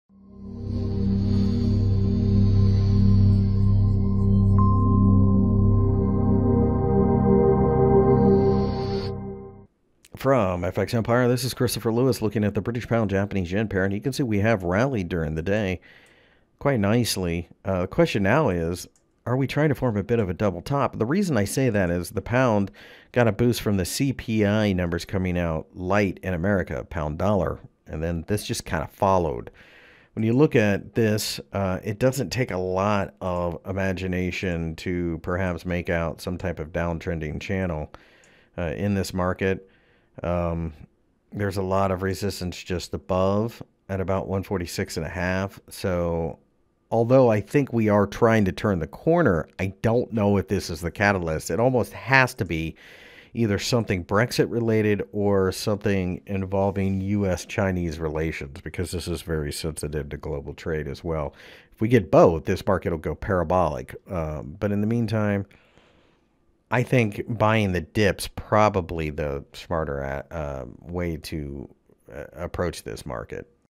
GBP/JPY Technical Analysis for September 14, 2018 by FXEmpire.com
The British pound rallied significantly against the Japanese yen on Thursday, reaching towards the ¥146 level. For the full article go - GBP/JPY Forecast September 14, 2018, Technical Analysis.Find more information about Forex and Commodities News Technical Analysis Fundamental Analysis and Brokers on FX Empire